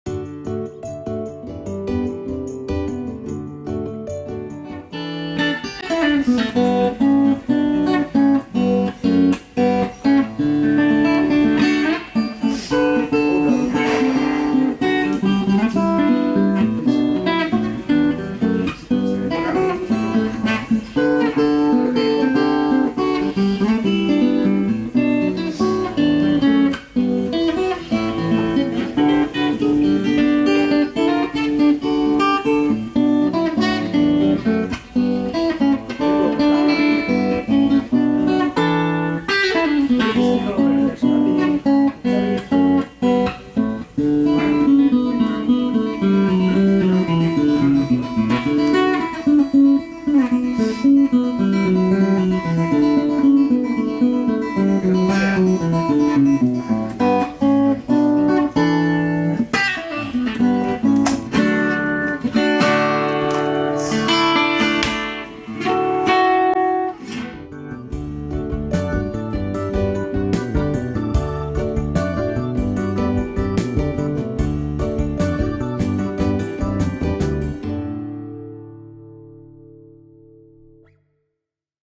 Baby Taylor BT2E Test through Fender Amp (Part.2)
This video contains a voice sample of Baby Taylor guitar belongs to Mr. Anang Hermansyah. He is Indonesia's top musician / songwriter / singer and producer. This guitar is the second of the four guitars that have been repaired in our workshop and we got a call from him yesterday saying he was very satisfied with the results of our work.

Sound sample of this guitar is divided into two separate videos, on the first video recorded directly by the video camera on my Lumia phone and second, recorded through the Fender guitar amps. Make sure you watch both videos.
And you can see the pics on my FB page

Robby Ramdhani,
Chakra Acoustic Guitars
Kreji Ukuleles